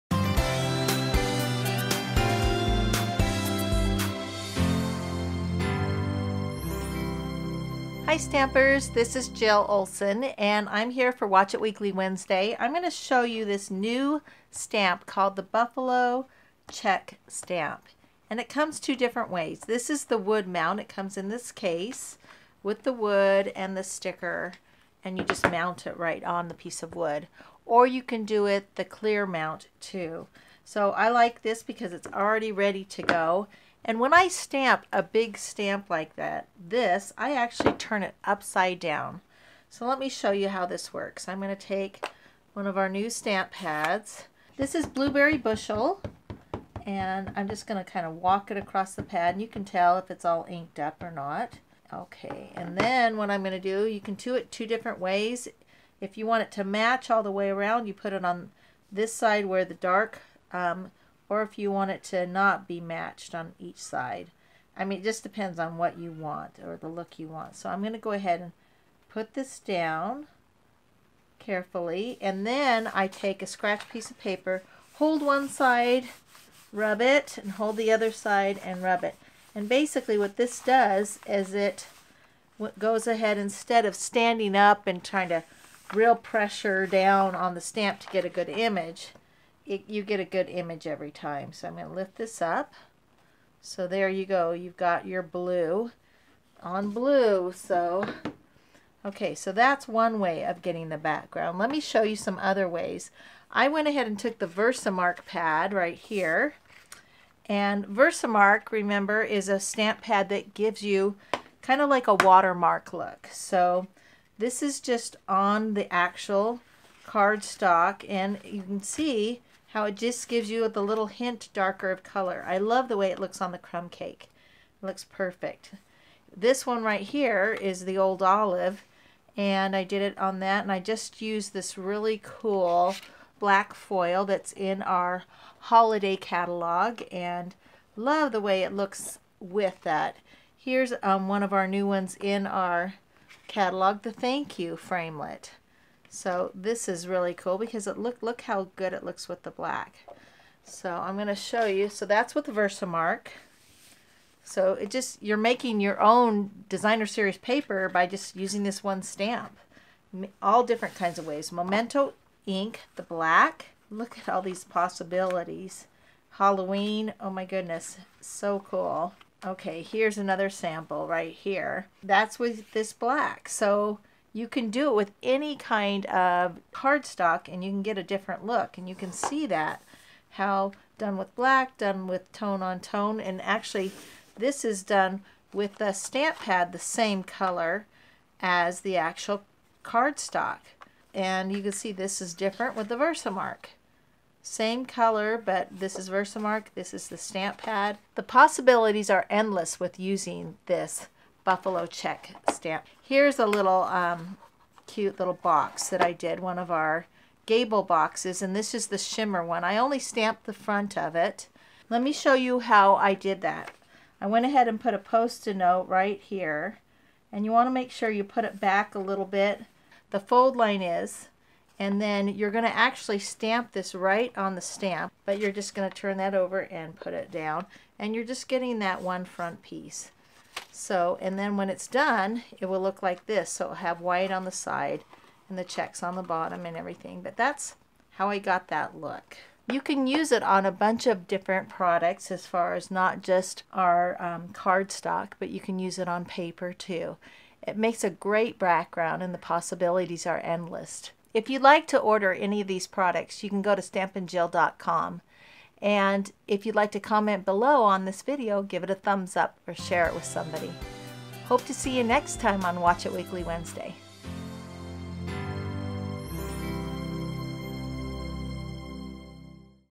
Stamping Jill - Buffalo Check Background Stamp Techniques & Card Ideas
Stampin' Up! Demonstrator Jill Olsen demonstrates the NEW Buffalo Check Background Stamp . You will see many techniques on how you can create different color backgrounds. The possibilities are endless, check it out!